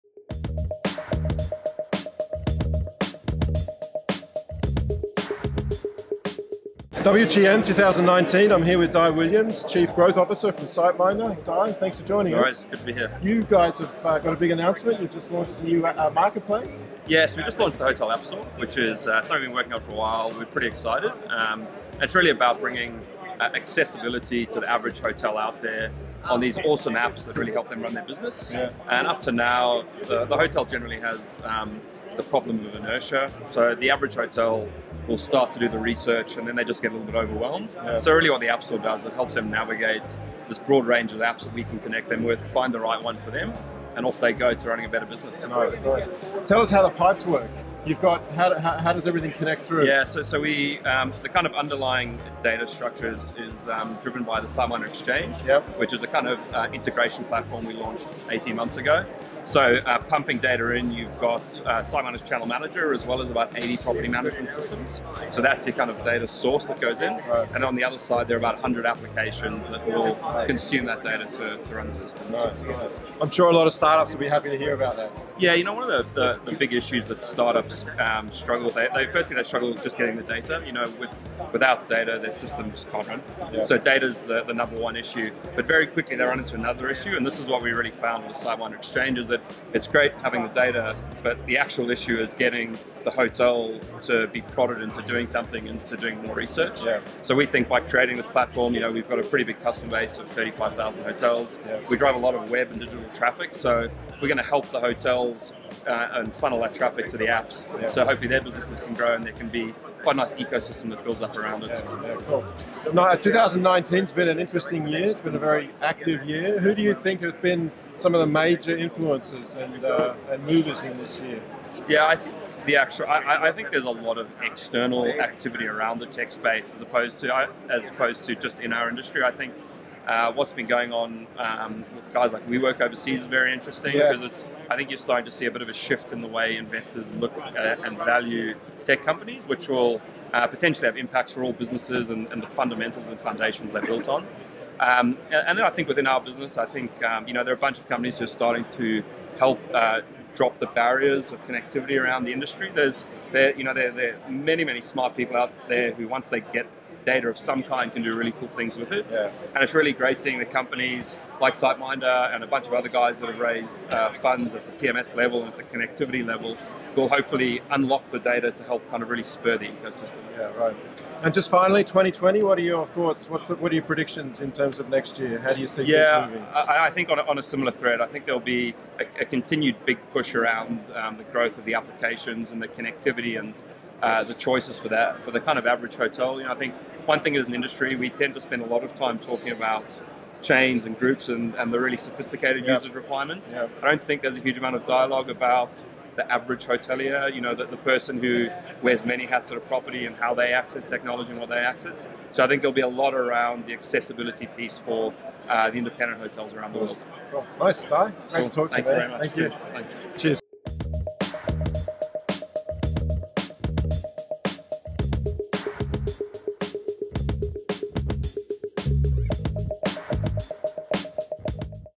WTM 2019 | Dai Williams - SiteMinder | Quick Chat
WTM 2019 is up and running - we chat quickly with Dai Williams on all the latest happenings with team SiteMinder.